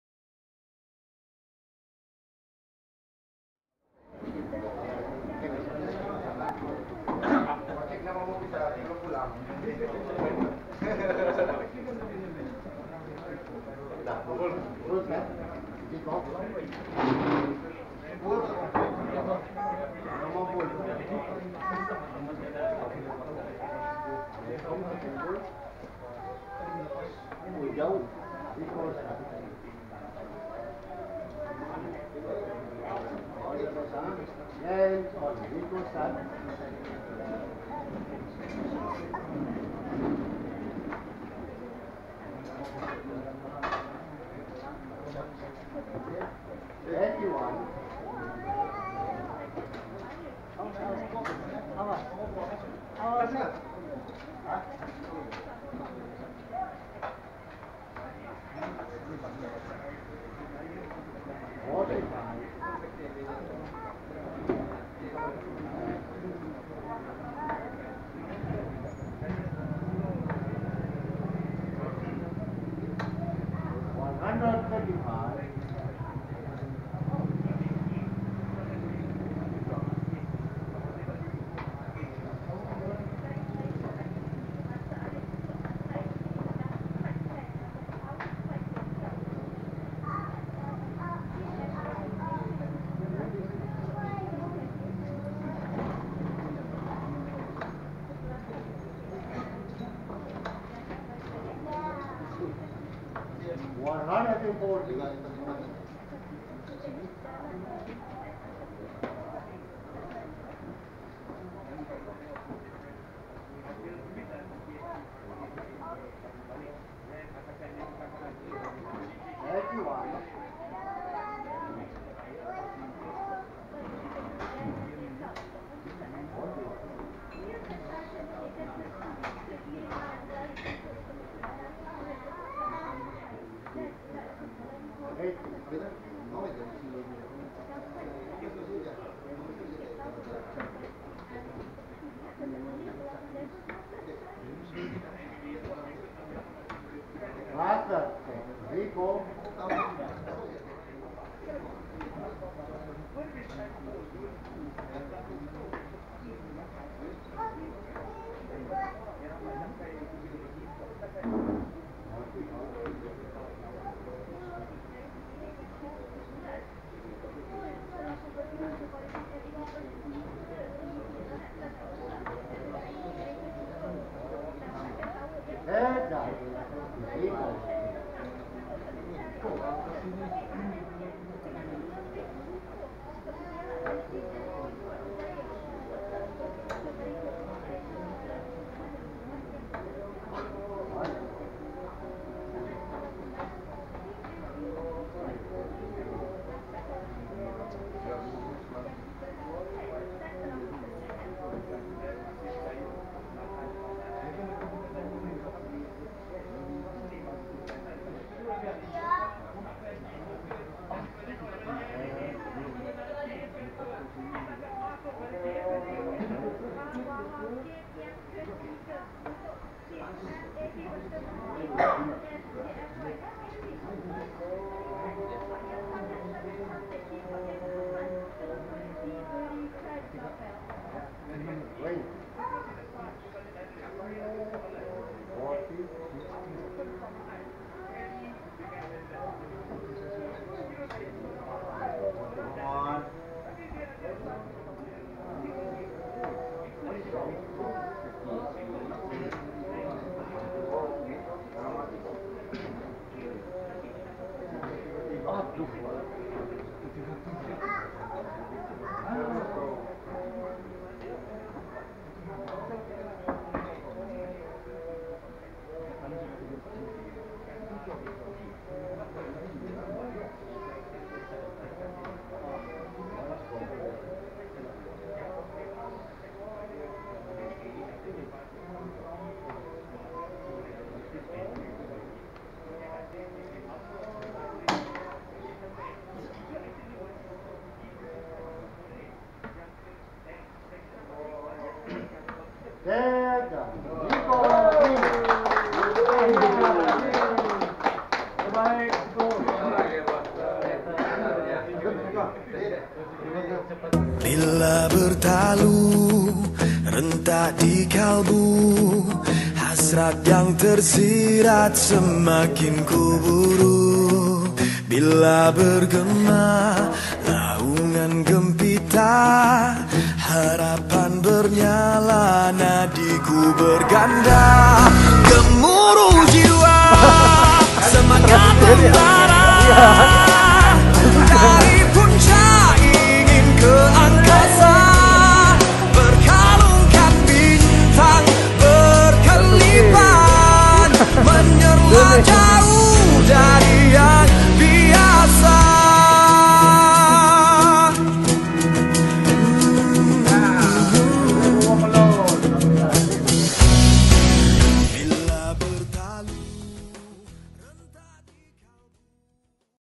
SANDAKAN DARTERS - FINAL ZICO VS MALCOM
Video Final pertandingan Mini tournament Sandakan Darters.